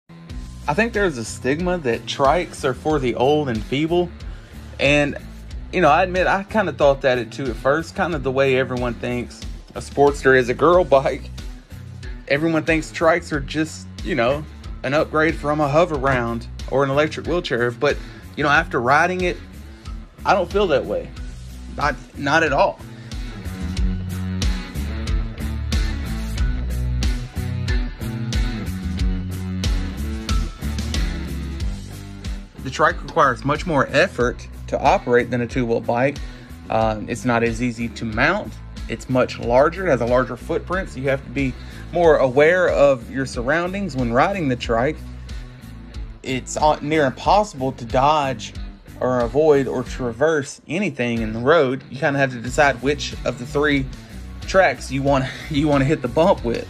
⚡FLASHBACK FRIDAY⚡Dwayne's 1st Time Riding a Harley Trike!
PART 2 - We discuss What the Differences are between Riding a 2 Wheel Harley & A Trike , and Some Advanced 3 Wheel Riding Techniques.  Dwayne Shares His Impressions of His First Trike Ride. 👍 Full Video
⬇️⬇️⬇️⬇️⬇️ Click Show More Below ⬇️⬇️⬇️⬇️⬇️

📸 Our Camera Gear

The DK Team